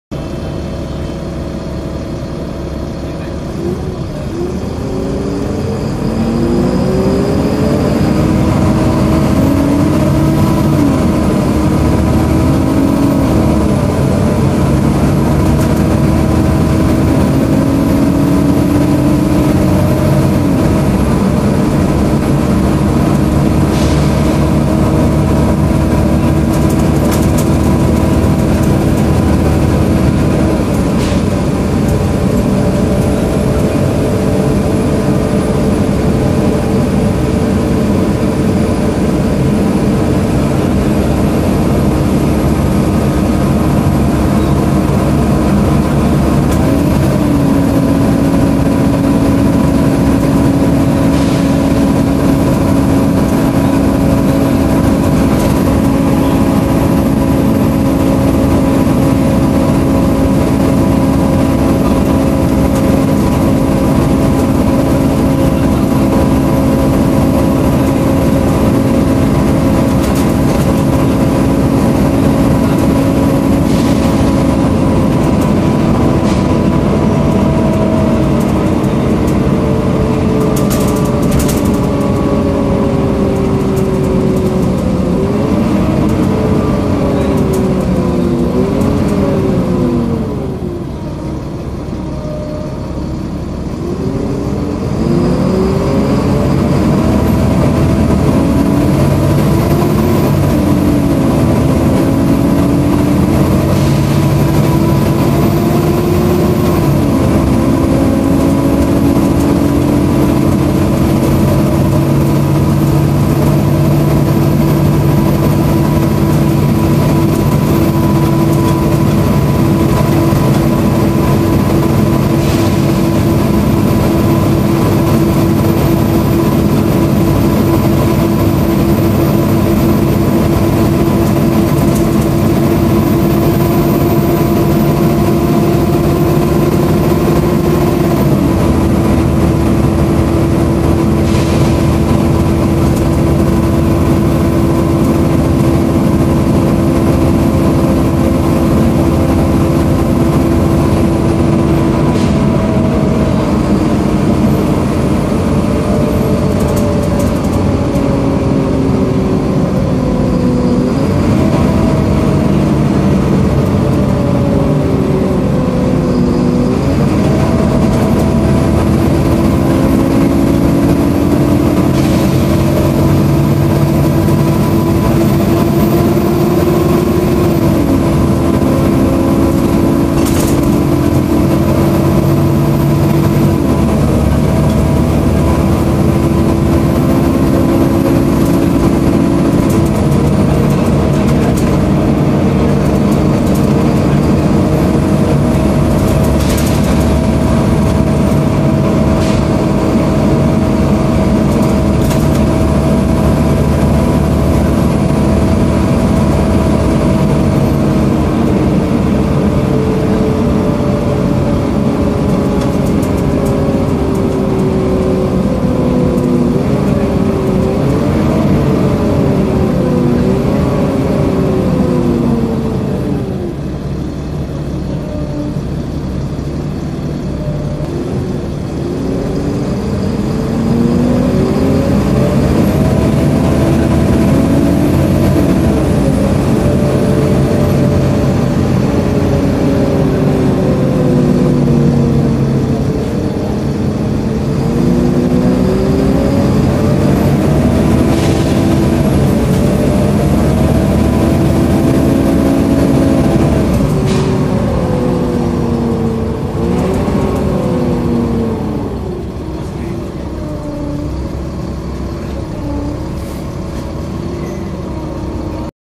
Service: 72 | First West Yorkshire Wright StreetCar 19005 (YK06 ATX) loud cooling fan
Volvo B7LA/Wright StreetCar
Service 72: Leeds - Armley - Stanningley - Thornbury - Bradford
Date filmed: 19th February 2016